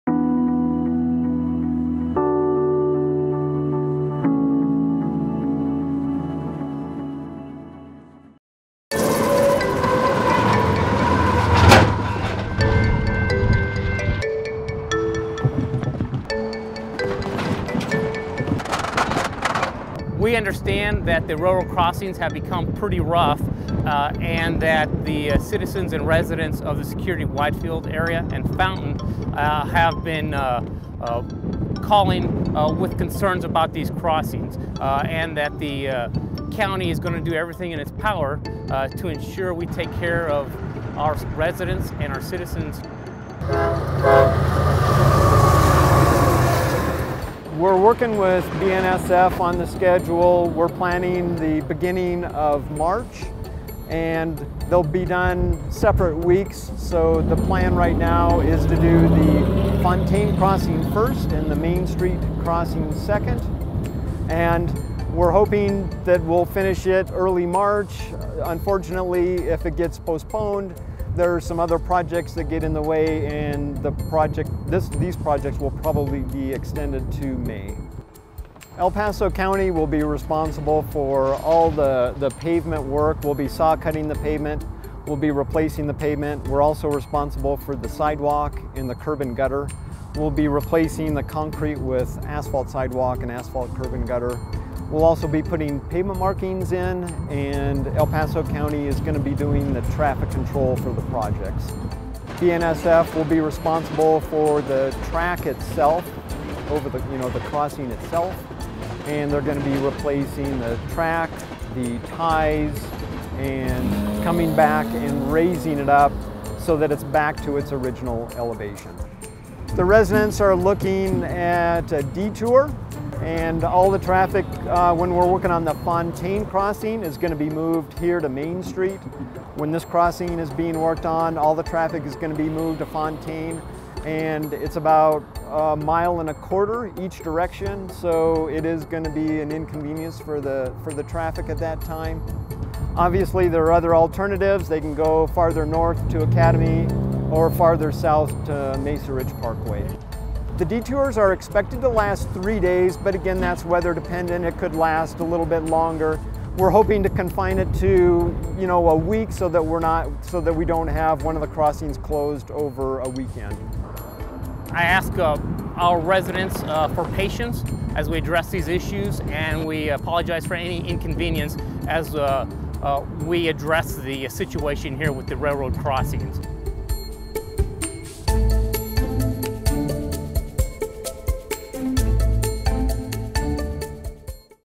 Railroad Crossings Repair Project
El Paso County and BNSF Railway will collaborate on the project that will mend tracks that have sunk a few inches and caused slowdowns at the Main Street and Fontaine Boulevard crossings just east of Colorado Highway 85/87 south of Academy Boulevard.